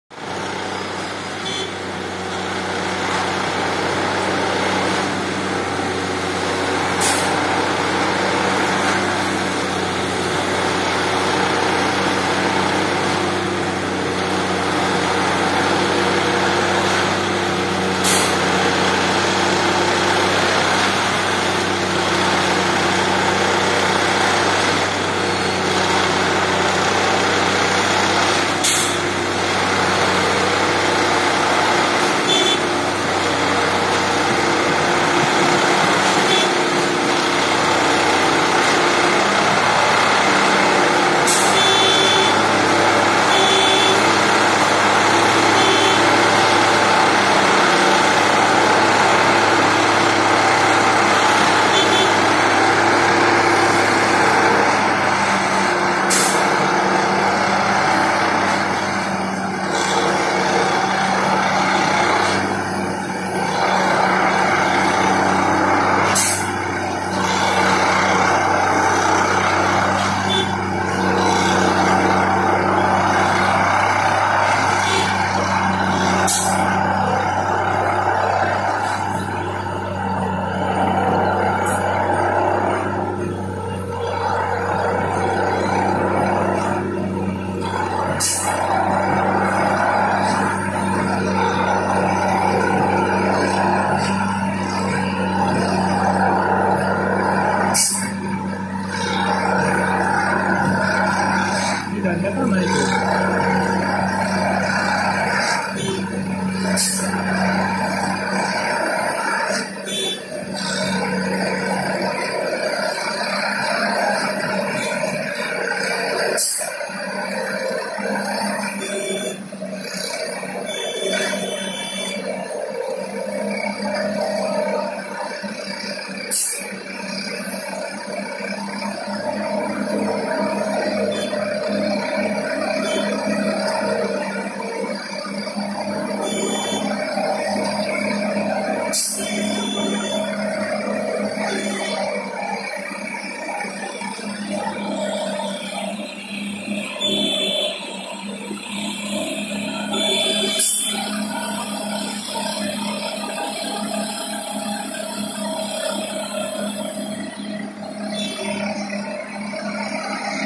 Track packing machine working on Chittaurgarh Jn. on newly laid down railway tracks...
10th Mar. 2019, 3.00 p.m.